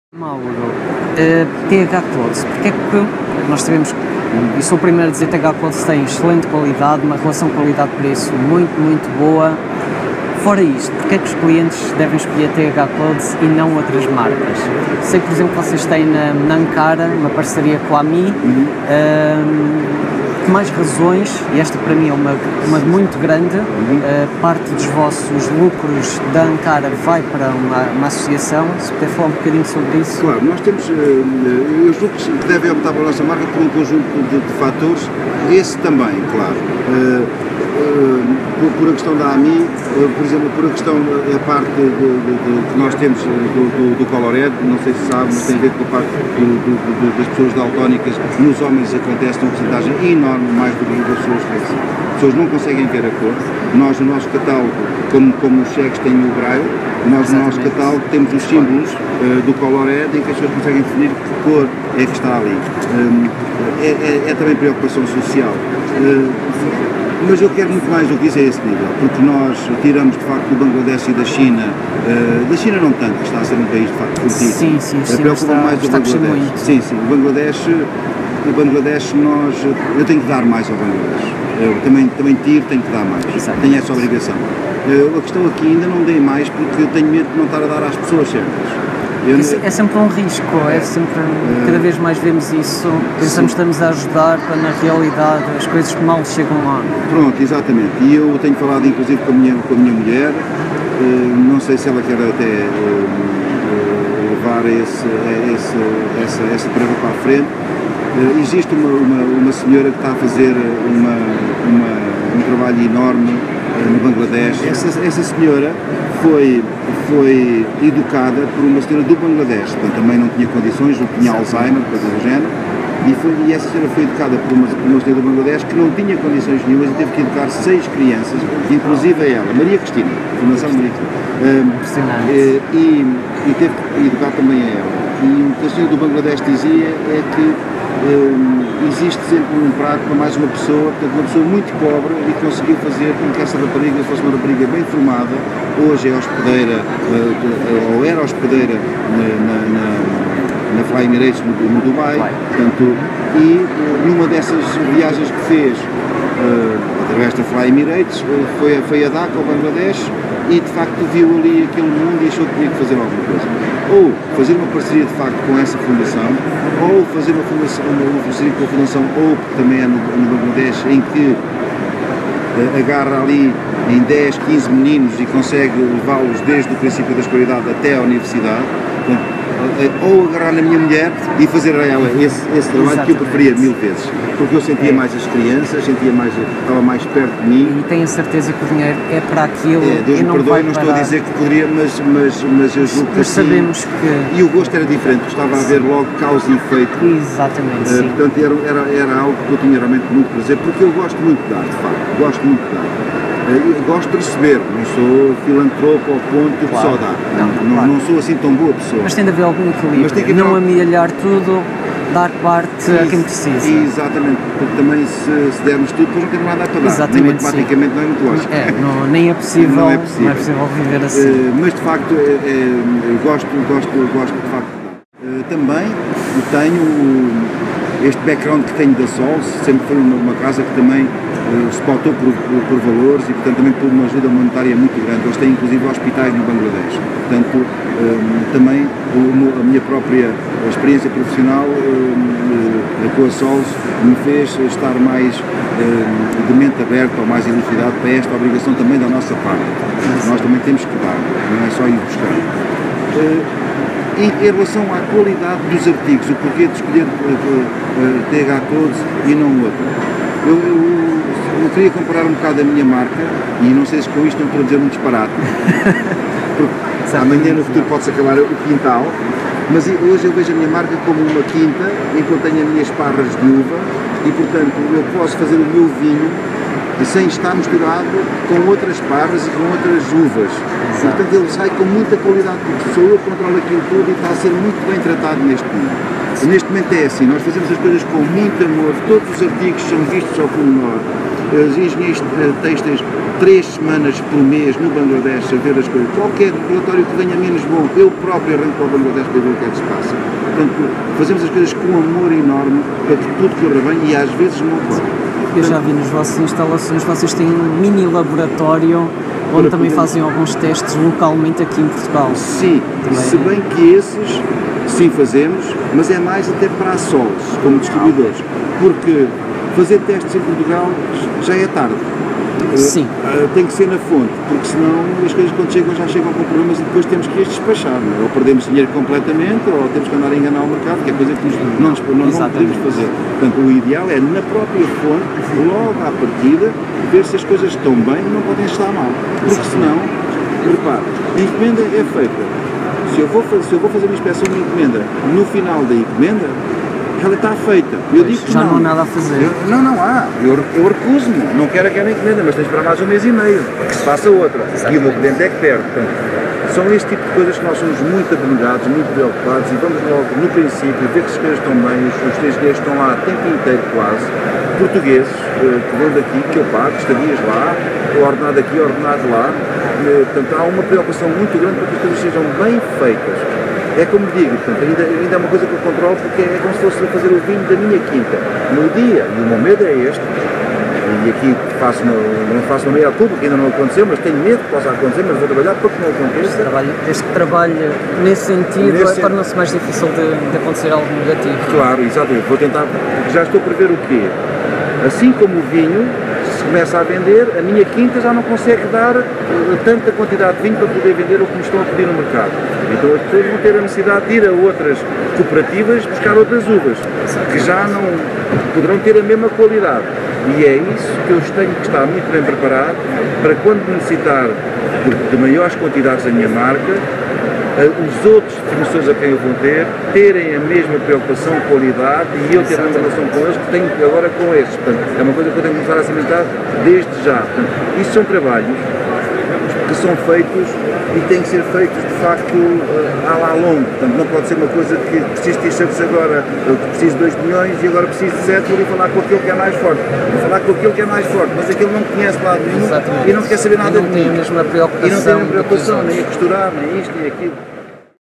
ENTREVISTA TH Clothes - Pt3 - O que diferencia a TH Clothes das outras marcas?
Hoje seguimos com a continuação ao Mauro da TH Clothes e tentamos descobrir o que diferencia a TH Clothes das restantes marcas de têxteis promocionais. Para pedidos de orçamento, preencha o nosso formulário